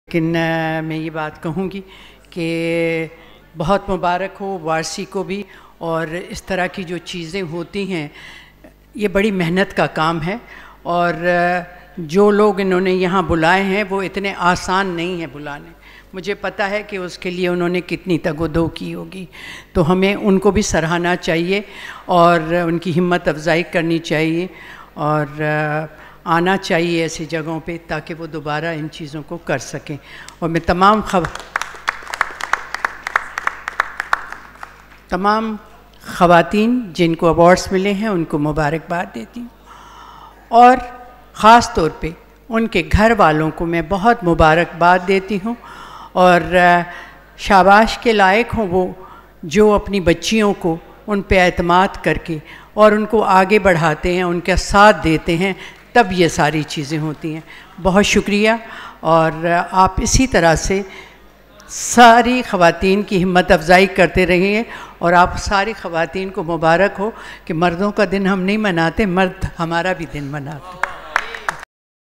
Guest Speaker CEO HUM TV Sultana Siddiqui, Guest of Honour Global Women's Award ®
All Copy Rights and Reserved by
WIO Official YouTube Channel®